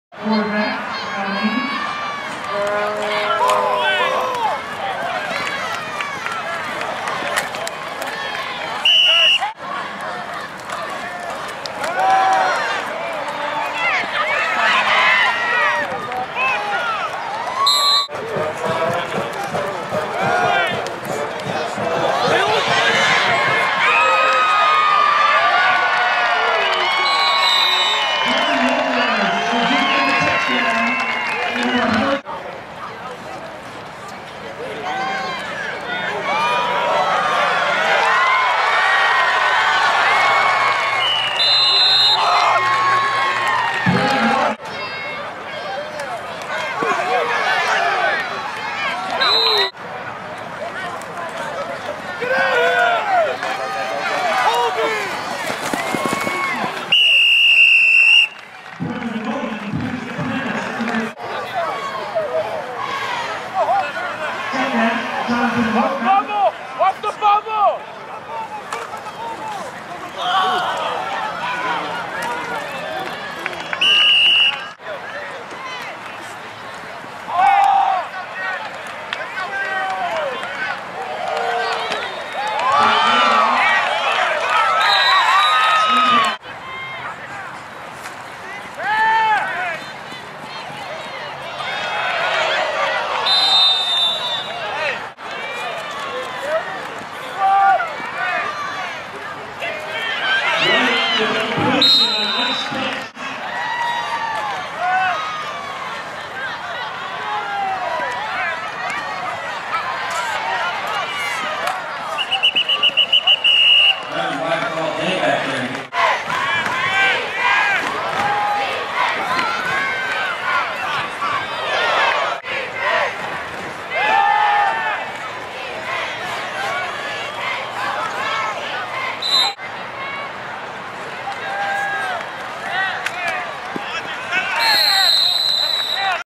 QB Brevin White - Alemany (CA) Class of '18
Watch High School Football America's highlights of Alemany (California) sophomore quarterback Brevin White against Colony on September 4, 2015.